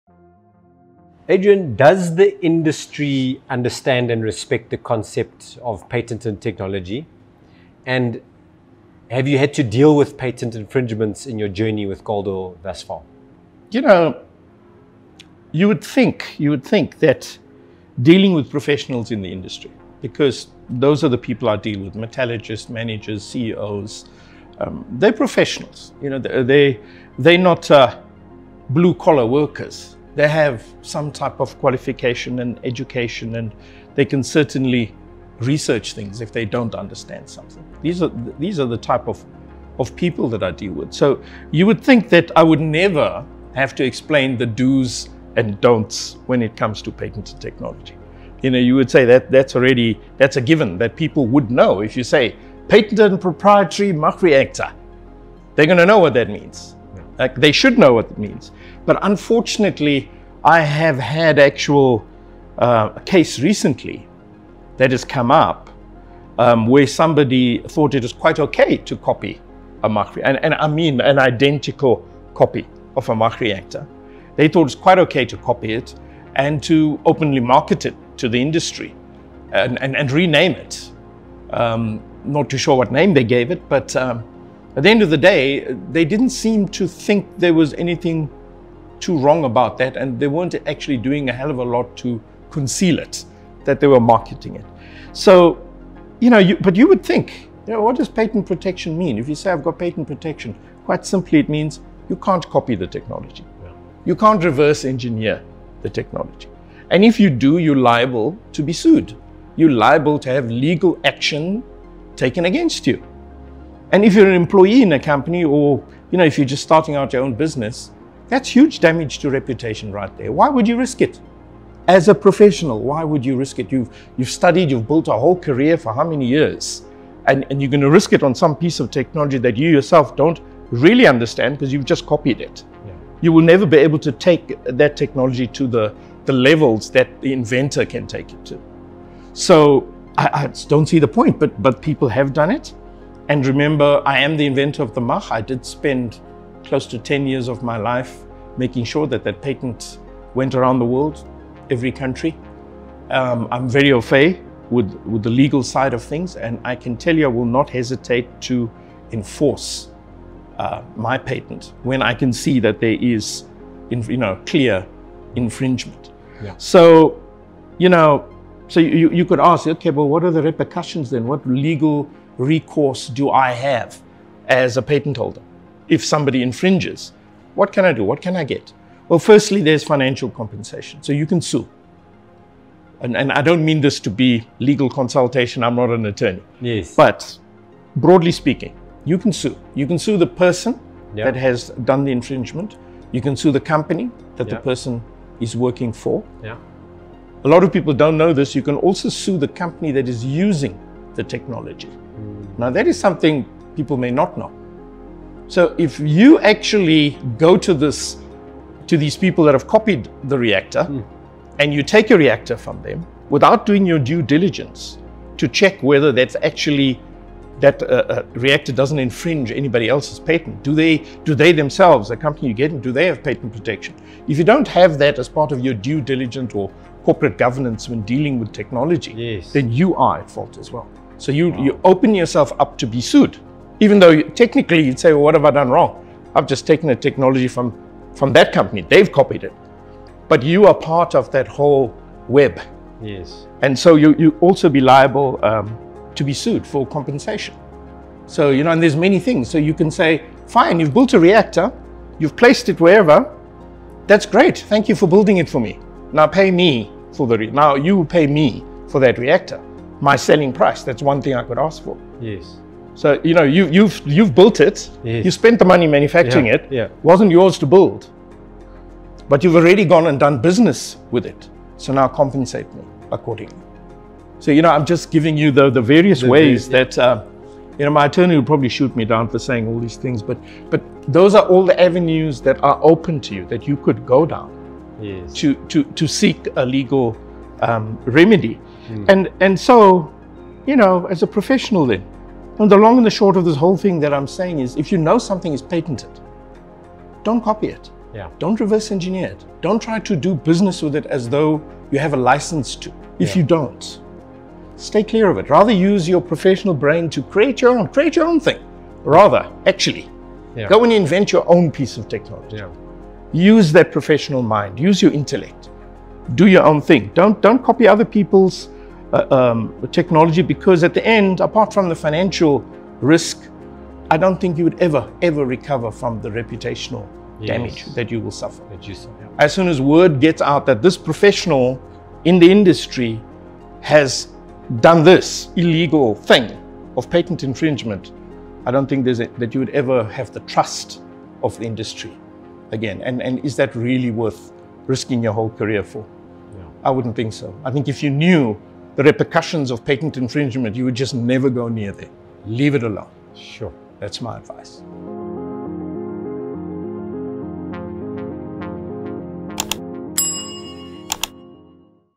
Listen in as we discuss embarking on the journey of patent protecting your property.
Listen in as we discuss the intricate details of embarking on the journey of patent protecting your property.

We’ll share how we have personally faced threats of theft from others attempting to steal the idea behind the revolutionary MACH REACTOR™ technology and how patent protection assisted in the situation. We also delve into what legal action can be taken if someone attempts to steal your patent protected property.

You won’t want to miss a second of this priceless information!